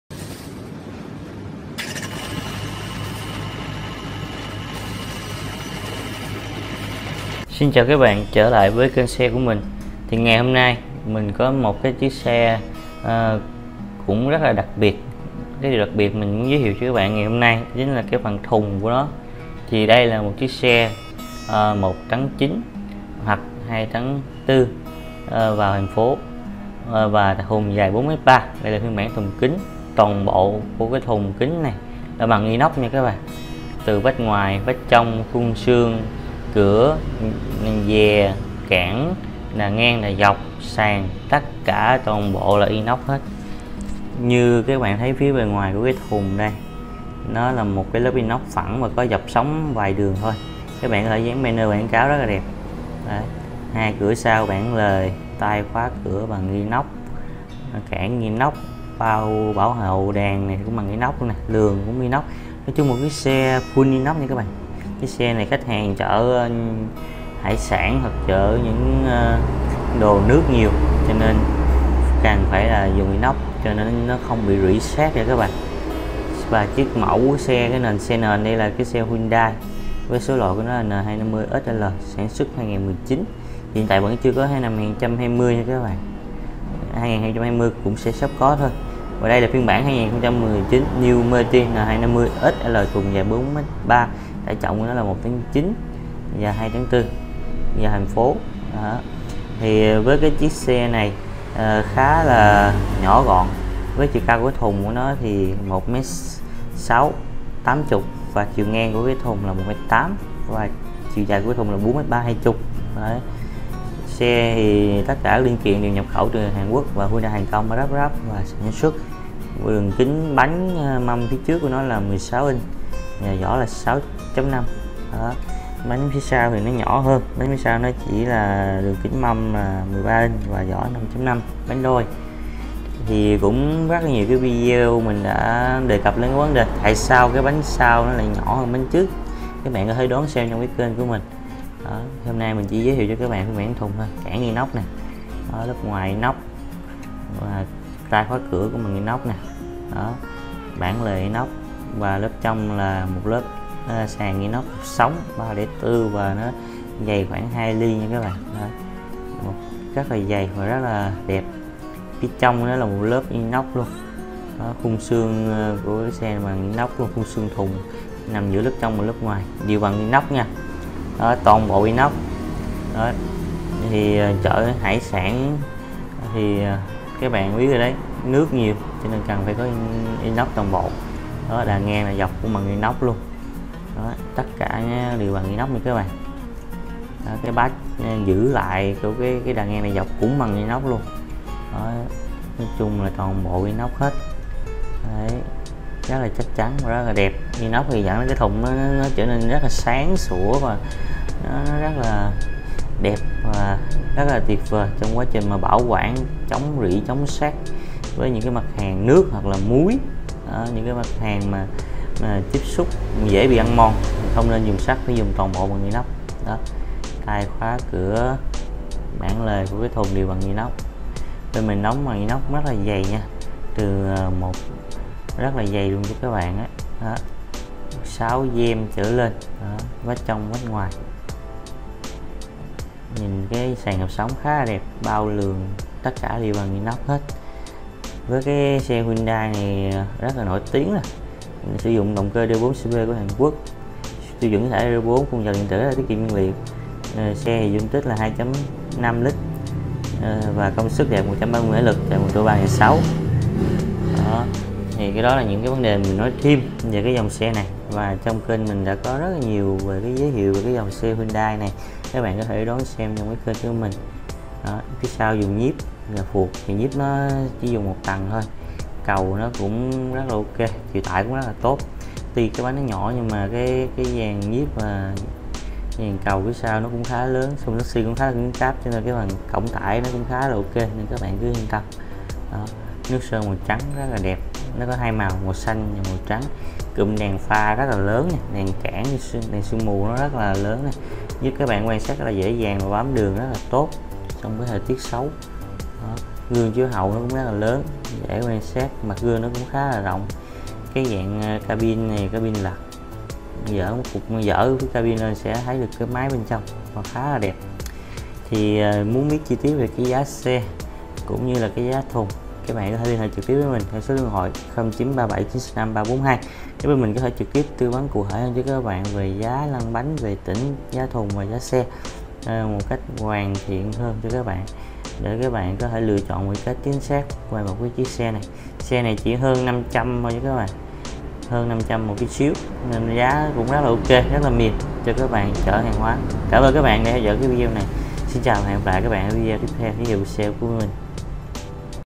✅Xe Tải Bình Dương - Xe tải Hyundai 1.9 Tấn N250SL thùng kín toàn bộ Inox siêu dài 4m3
Thông số chung xe Hyundai N250SL
Trọng lượng bản thân: 2450 kG
Phân bố :
- Cầu trước: 1400 kG
- Cầu sau: 1050 kG
Tải trọng cho phép chở: 2350 kG
Số người cho phép chở: 3 người
Trọng lượng toàn bộ: 4995 kG
Kích thước xe: Dài x Rộng x Cao: 6250 x 1900 x 2560 mm
Kích thước lòng thùng hàng (hoặc kích thước bao xi téc): 4250 x 1850 x 1700/--- mm
Khoảng cách trục: 3310 mm
Vết bánh xe trước / sau: 1485/1275 mm
Số trục: 2
Công thức bánh xe: 4 x 2
Loại nhiên liệu: Diesel
Động cơ
Nhãn hiệu động cơ: D4CB
Loại động cơ: 4 kỳ, 4 xi lanh thẳng hàng, tăng áp
Thể tích: 2497 cm3
Công suất lớn nhất /tốc độ quay: 95.6 kW/ 3800 v/ph
Lốp xe
Số lượng lốp trên trục I/II/III/IV: 02/04/---/---/---
Lốp trước / sau: 6.50R16 /5.50R13
Hệ thống phanh
Phanh trước /Dẫn động: Phanh đĩa /Thuỷ lực, trợ lực chân không
Phanh sau /Dẫn động: Tang trống /Thuỷ lực, trợ lực chân không
Phanh tay /Dẫn động: Tác động lên bánh xe trục 2 /Cơ khí
Hệ thống lái
Kiểu hệ thống lái /Dẫn động: Trục vít - ê cu bi /Cơ khí có trợ lực thuỷ lực
Hyundai_N250_SL hyundai n250 sl